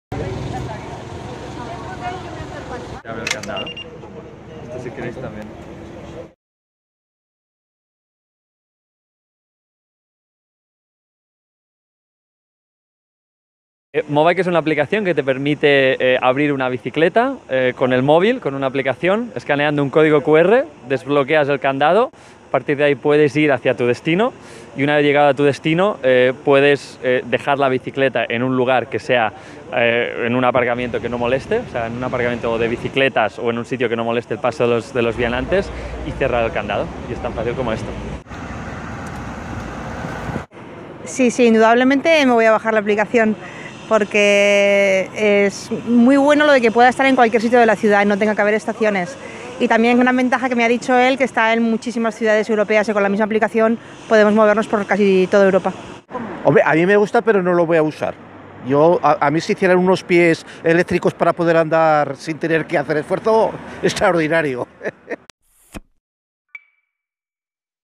Zaragoza estrena un nuevo servicio de alquiler de bicis
La empresa Mobike pone en marcha en Zaragoza un nuevo servicio de alquiler de bicis sin estación. Funcionan mediante una app y son 300 las que se pueden utilizar en la capital aragonesa con un coste de un euro por 20 minutos.